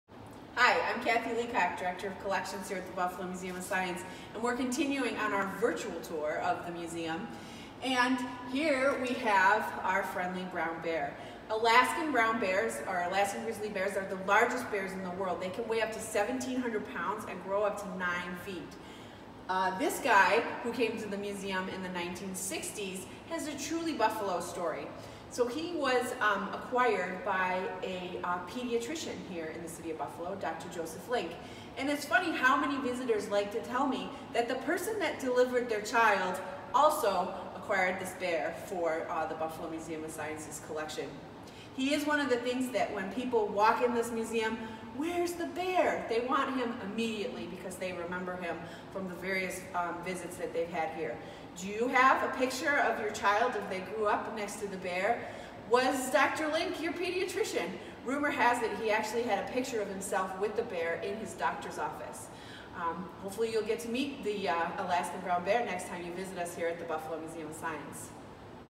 Buffalo Museum of Science Virtual Tour: Brown Bear
Let’s continue our virtual tour of the Museum by featuring one of our tallest residents. Our friendly Alaskan brown bear is one of our most asked-about Collection pieces and our Director of Collection’s stories will tell you why!